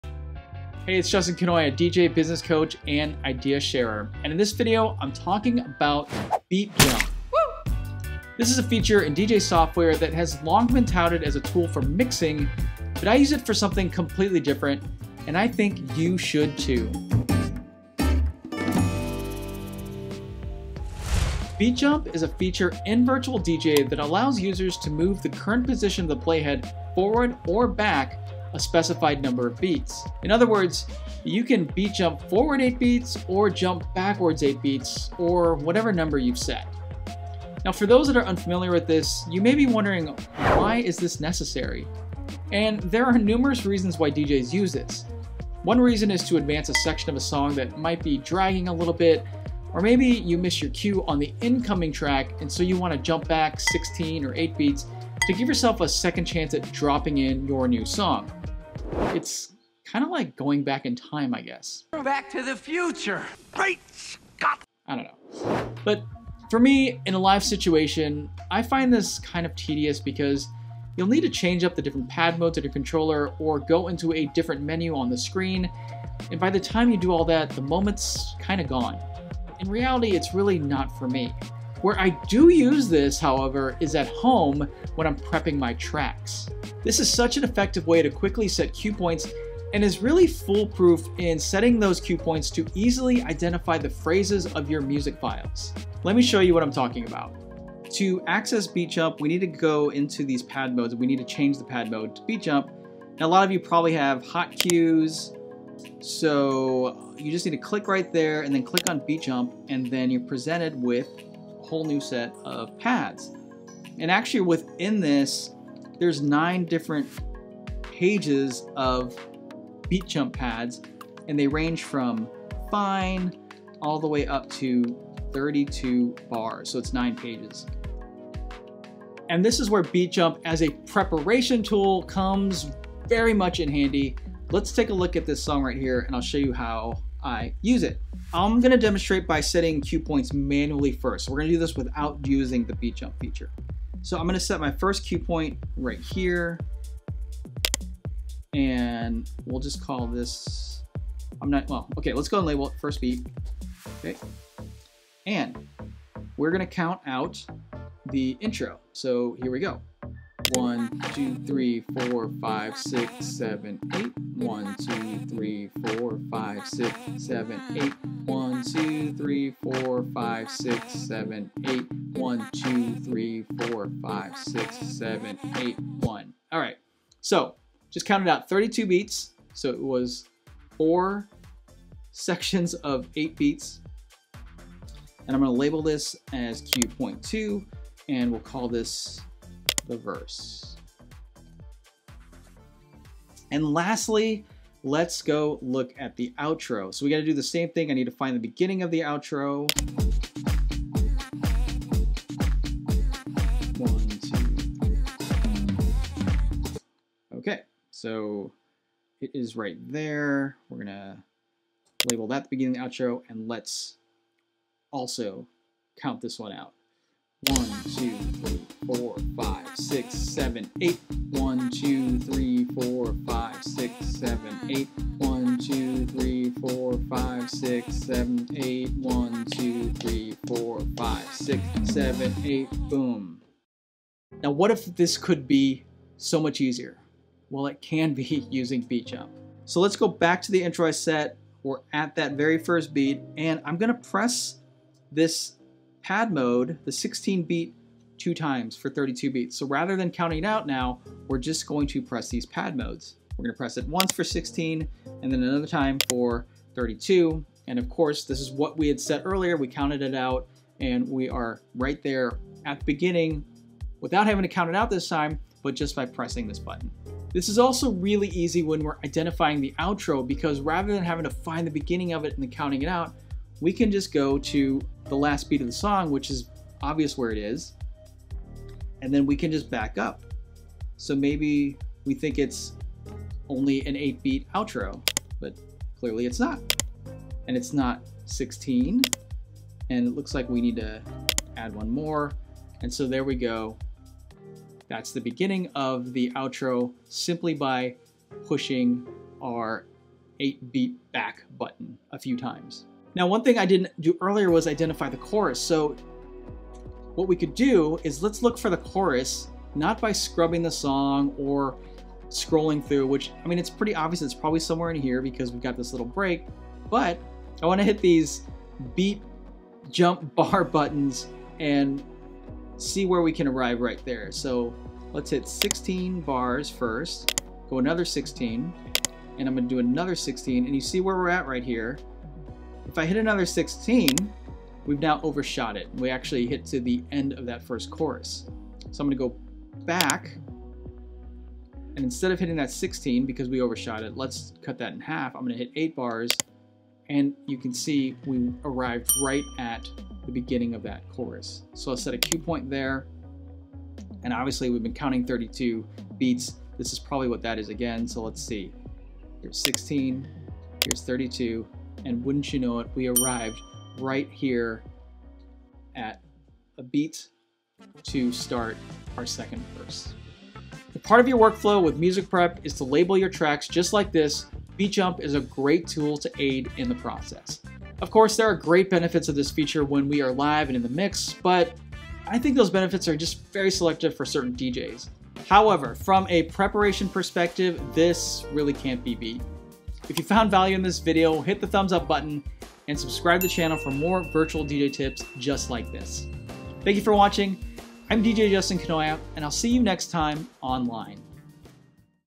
Virtual DJ Tutorial - Using Beat Jump For Track Prep (SET CUE POINTS QUICKLY)
Download my free Virtual DJ Guide: Justin’s 10 Tips and Tweaks

Virtual DJ Tutorial - Using Beat Jump For Track Prep In my latest Virtual DJ tips and tricks video, I'm sharing my secret hack in track preparation and how I set cue points on my DJ music efficiently.

Beat Jump is a feature that is used regularly for creative mixing and enabling DJs to quickly jump around different parts of a song. But I actually like to use this feature when I am setting my cue points and prepping my sets and music. I'll show you how I do this by activating the beat jump pad mode within Virtual DJ.

There is much to learn for Virtual DJ beginners or anyone looking for a Virtual DJ tutorial.